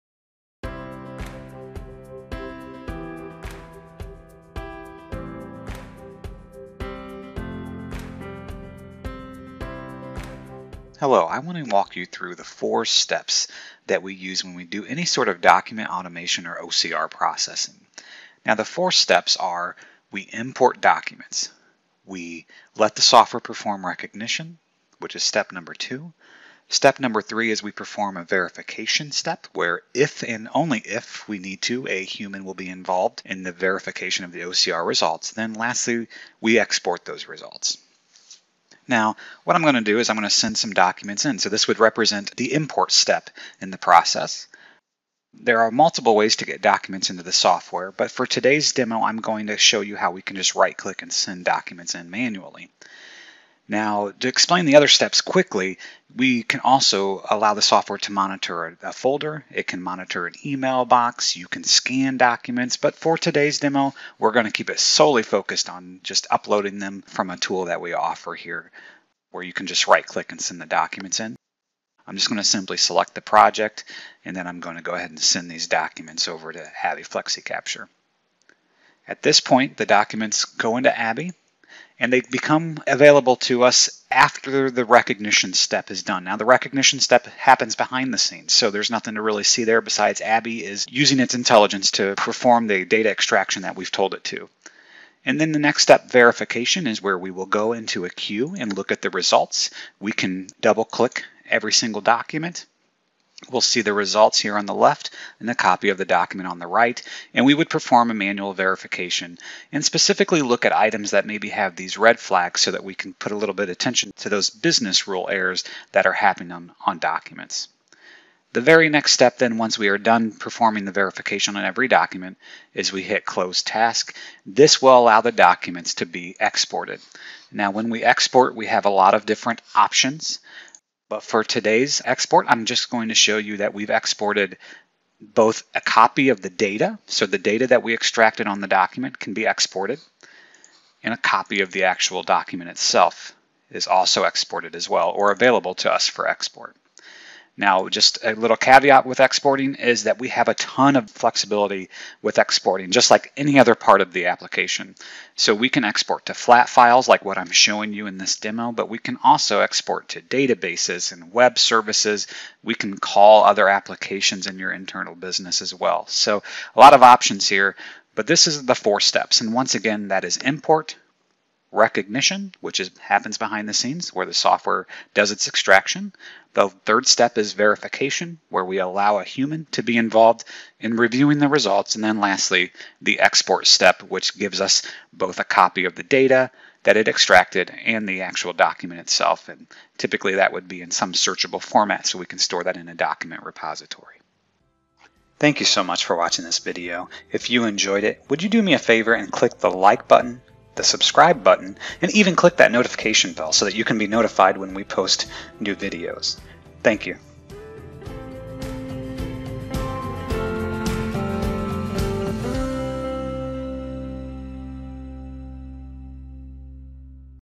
ABBYY FlexiCapture 12- Four Simple Steps of OCR Processing
Discover the Four steps of OCR Processing described in this video by Travis from @Ufcinc as well as a general overview of their functions. The four steps that occur in @ABBYYVIDEOS FlexiCapture are Import Documents, Software Recognition, Verification, and Export.

To find out more about UFC, Inc and the ABBYY FlexiCapture software please visit our